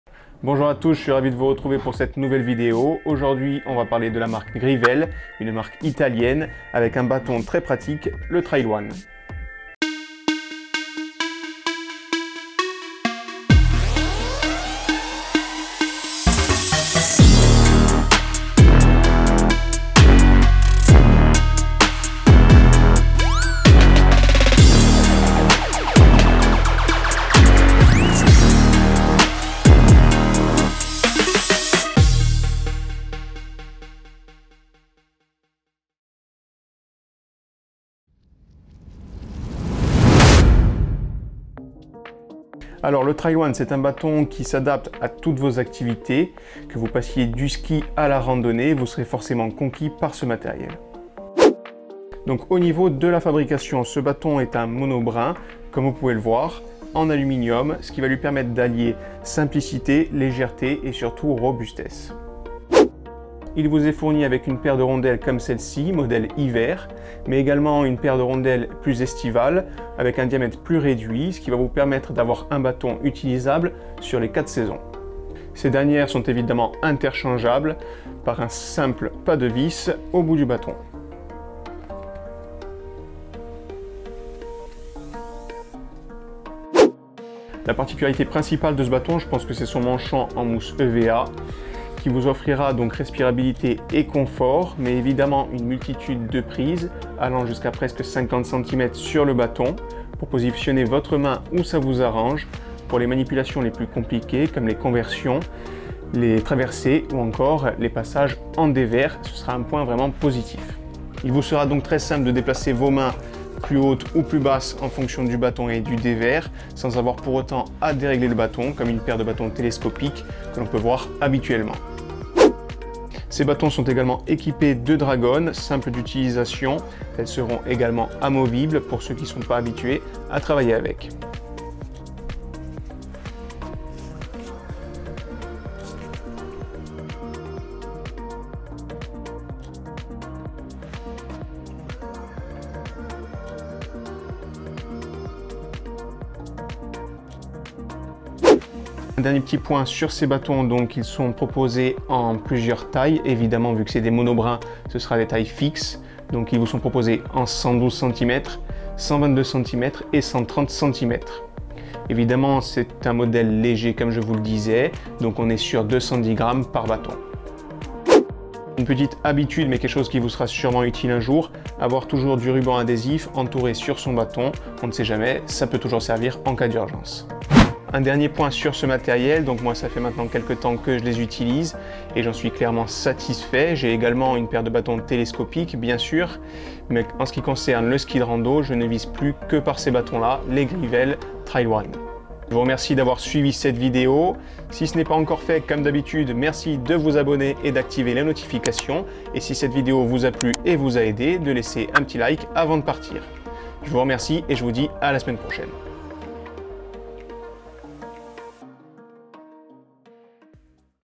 GRIVEL TRAIL ONE, des bâtons parfait pour le ski de rando
PREMIER EPISODE DE NOS RANDONNEES

ON VOUS PRESENTE NOTRE PACROUS ET ON VOUS FAIT PARTAGER NOS PAYSAGES

Dans cette vidéo on vous présente nos bâtons de ski de rando, de la marque GRIVEL, les TRAIL ONE .

Les bâtons Grivel Trail sont une gamme innovante de bâtons polyvalents, résistants et parfaits pour les 4 saisons. Adaptés à de nombreuses activités en montagne, du trekking au running et en passant par presque tous les styles de ski. Tous les corps des bâtons Grivel sont conçus en aluminium 7075 pour une résistance optimale à la relation de poids. Ils incluent tous deux rosettes d'été et d'hiver. Cette gamme dispose d'une poignée avec une large zone de mousse EVA ergonomique pour plus de confort et de respirabilité avec une dragonne amovible facilement adaptable. Les bâtons Trail 1 disposent d'une section pour un maximum de résistance et un poids minimum, sa poignée aux grandes dimensions en mousse EVA ergonomique vous offre un maximum d'adhérence et de respirabilité.

Caractéristiques :
- Dragonne amovible ajustable
- Poignée aux grandes dimensions
- Poignée en caoutchouc EVA ergonomique

    N'HESITEZ PAS A NOUS LAISSER VOS QUESTIONS EN COMMENTAIRE

                                   NOUS SERONS RAVIS D'Y REPONDRE